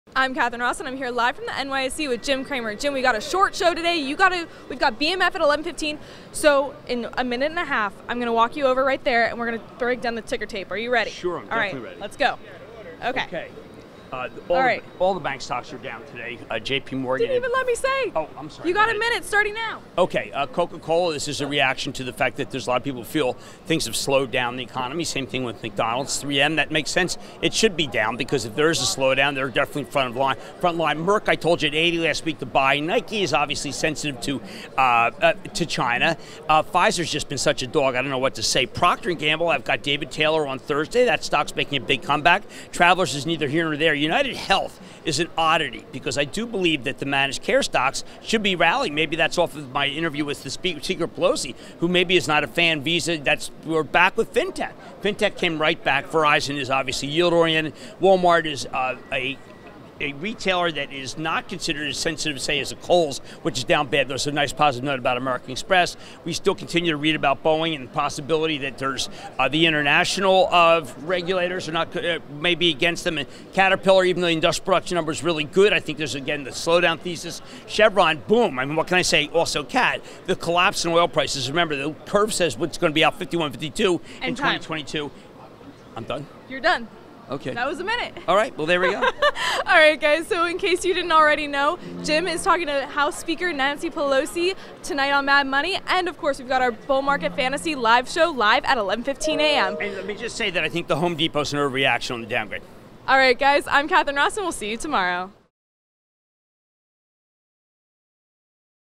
Got a Minute? Jim Cramer Breaks Down the Markets and Ticker Tape
Jim Cramer's breaking down the ticker tape for investors.

Sign up for ActionAlertsPlus.com today for exclusive insight into Jim Cramer’s charitable portfolio